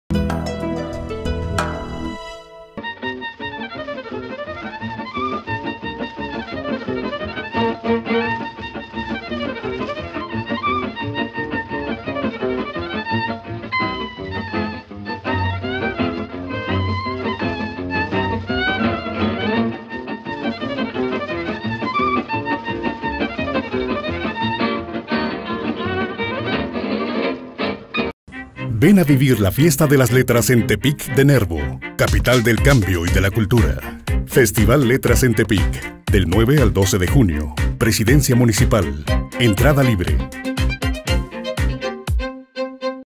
Martín Solares te invita a vivir la fiesta de las letras en Tepic de Nervo
Es autor de Los minutos negros y No manden Flores. Editor de Almadía, forma parte del consejo editorial de la revista Revuelta. Premio Nacional de Literatura Efraín Huerta 1998 por la novela fantástica breve El centro de la ansiedad. Premio Nacional de Cuento Infantil Juan de la Cabada 2008 por Monstruos. Parte de su obra se encuentra en antologías como Día de muertos y La littérature mexicaine des treinta dernières annes. Ha colaborado en La Jornada, Milenio, Proceso, Replicante y la página electrónica sololiteratura.com, entre otras publicaciones.

Del 9 al 12 de junio en la presidencia municipal. Entrada libre. ‪#‎FLTepic‬ 2016 ‪#‎CámbiaLEE‬